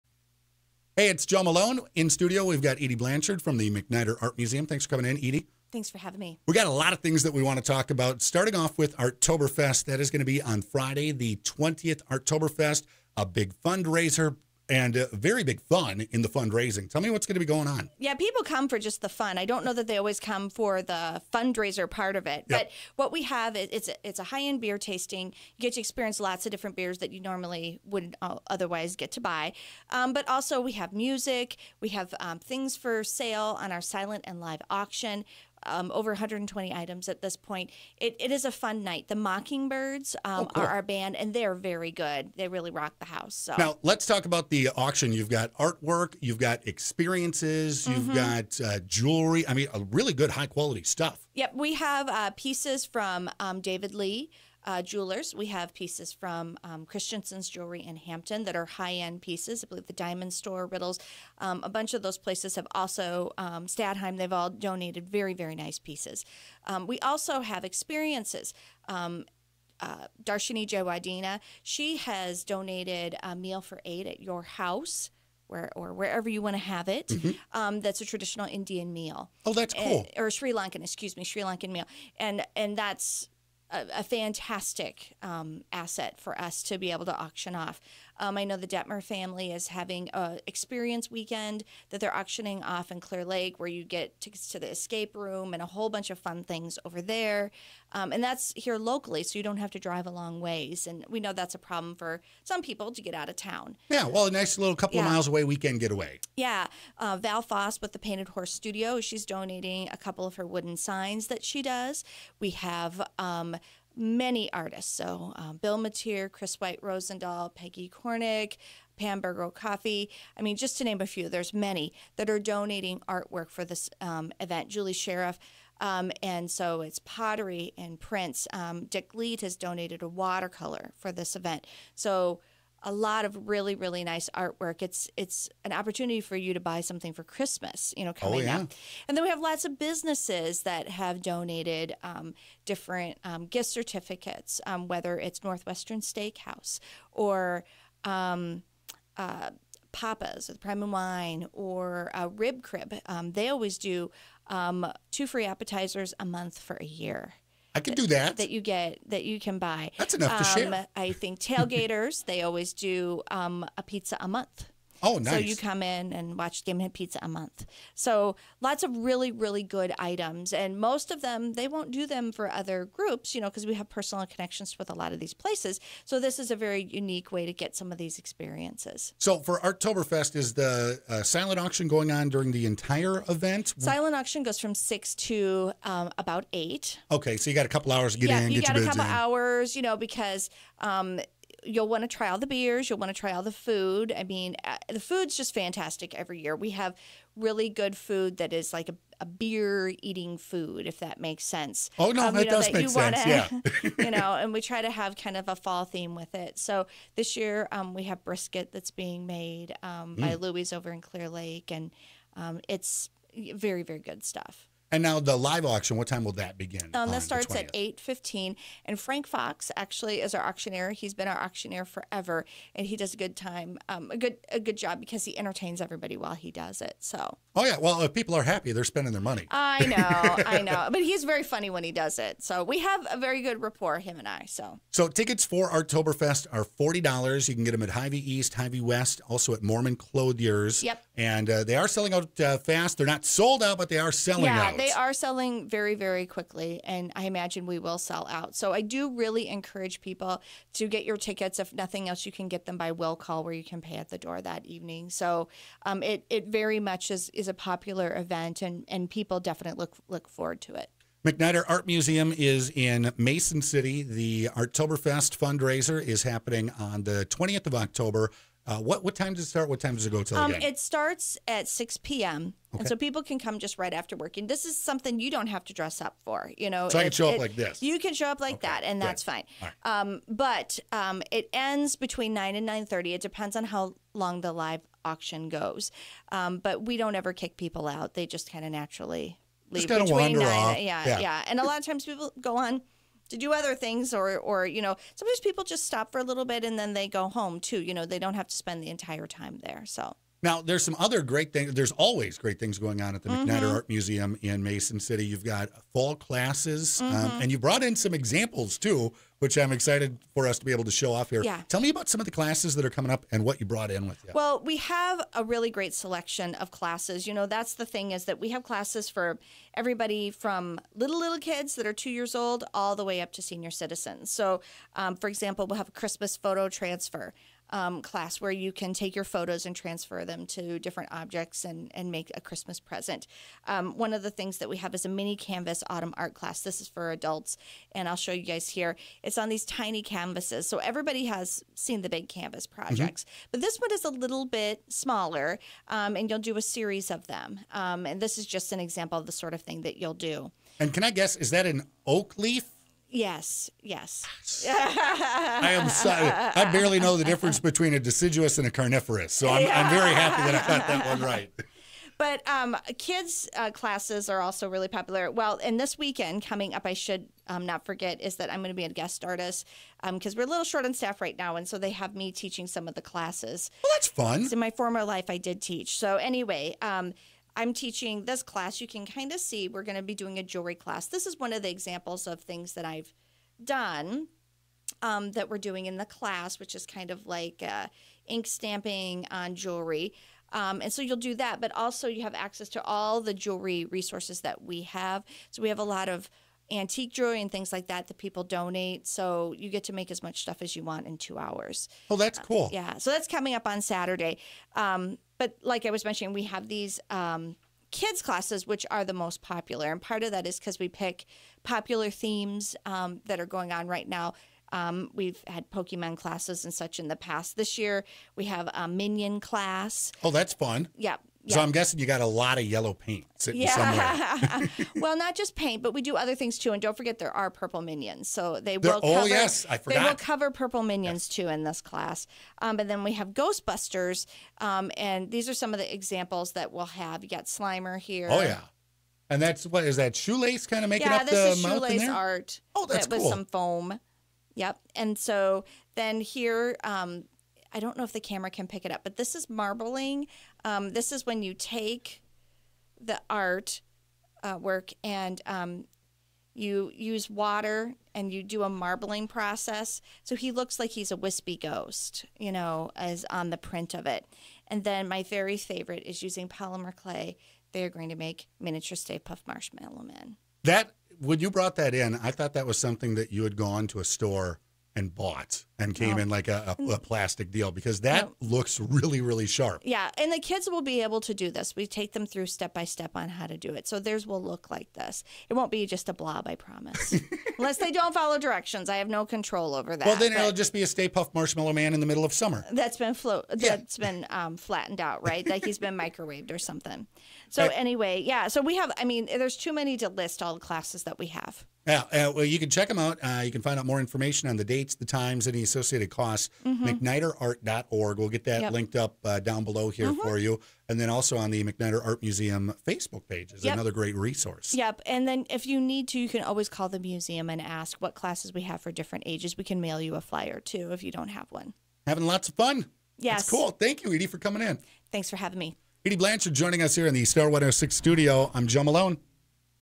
MacNider Artoberfest and classes
Edie Blanchard joins The Joe Show to talk about Artoberfest and the upcoming classes at the MacNider Art Museum in Mason City.  Visit macniderart.org